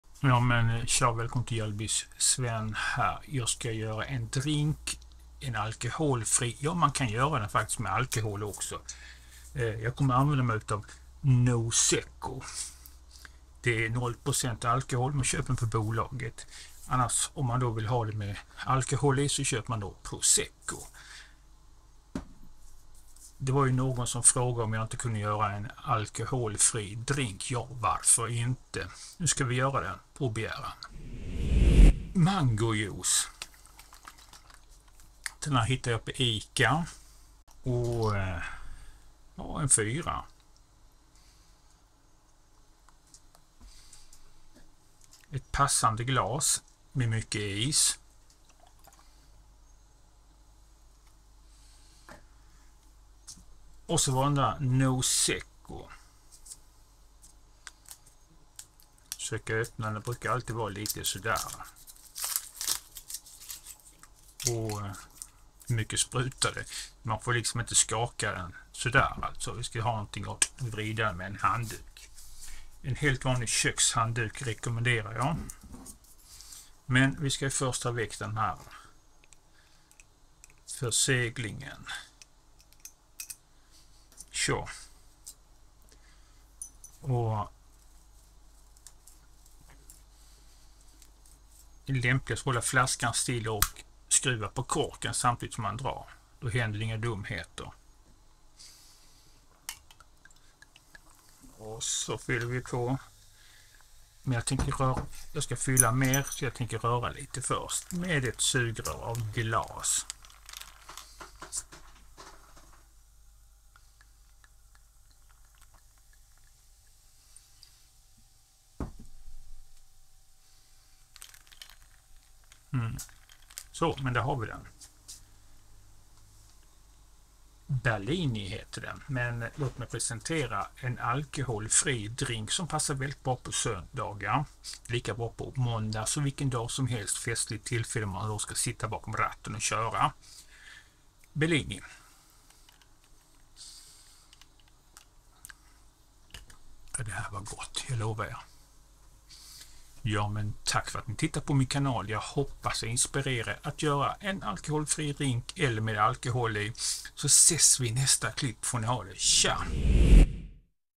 Falsk Bellini som alkoholfri drink | På begäran
Hej och välkommen till min kanal
Idag ska vi göra en känd proseccodrink fast som alkoholfri
Man tar Nozecco istället för Prosecco

Om någon vill komma ikontakt med mig.
Företag som vill göra samarbeten med mig.